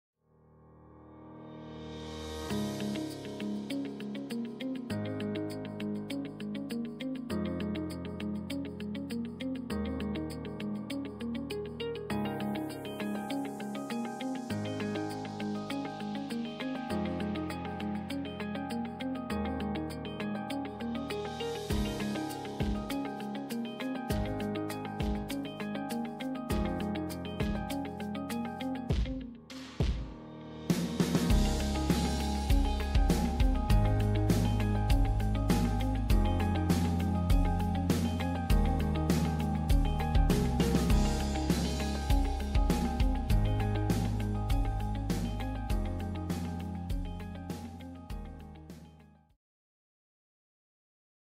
CO2 Galvo small letters engraving I 60W DAVI air cooled with 100*100mm working area
For this CO2 galvo laser, we tested letter height 5mm, 4mm and 2mm. It engraves very clear and nice.

CNC machine, laser machine supplier!